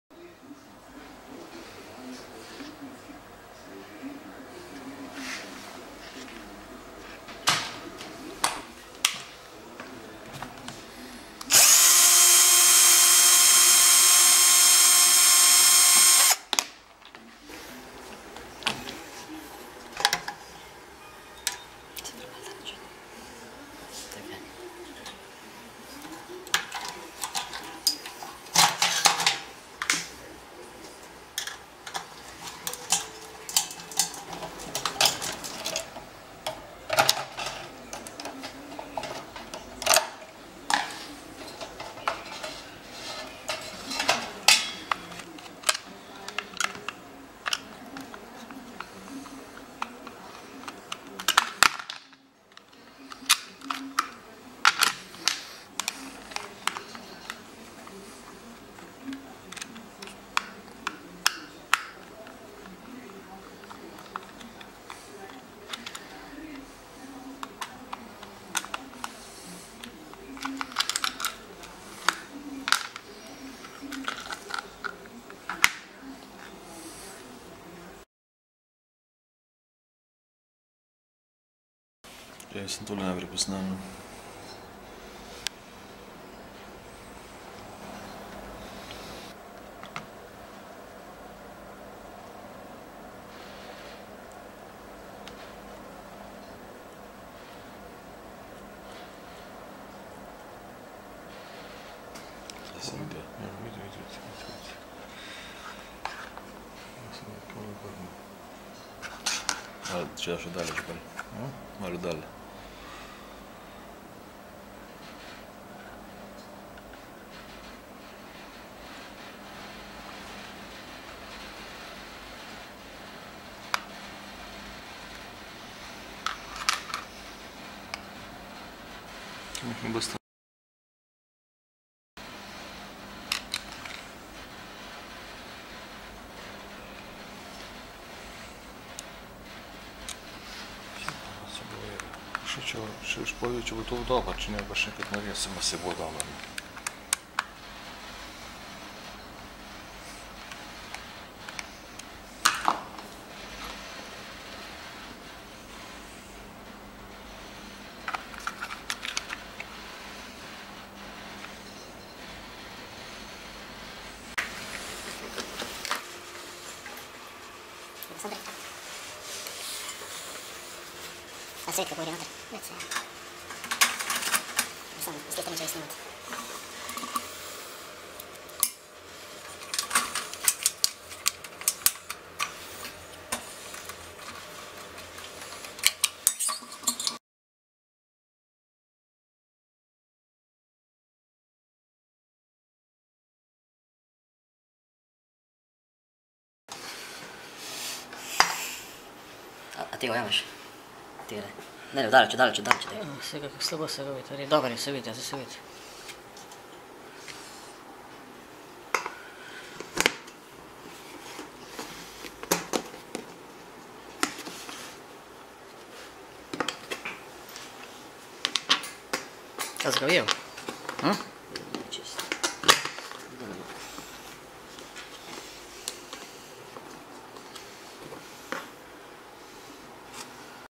Milky Mini Pasteurizer FJ 15 Service
This is a short video for unscrewing and assembling the control unit of our FJ 15 Mini Pasteurizer. This is just to check if everything is alright with the control unit.